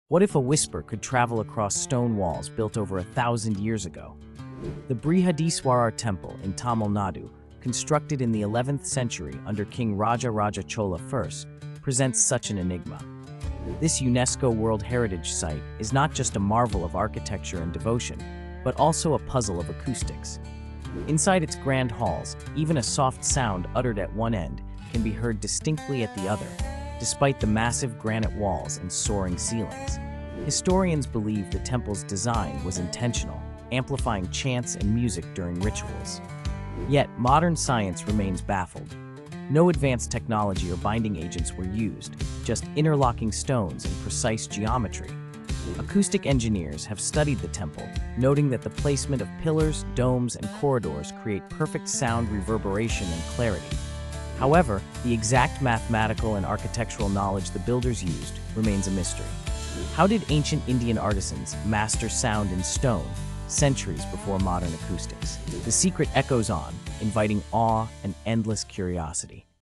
Unraveling Brihadeeswarar Temple's Sound Secrets
How did ancient Indian builders create acoustic marvels within the Brihadeeswarar Temple? Discover the mysteries that still puzzle scientists!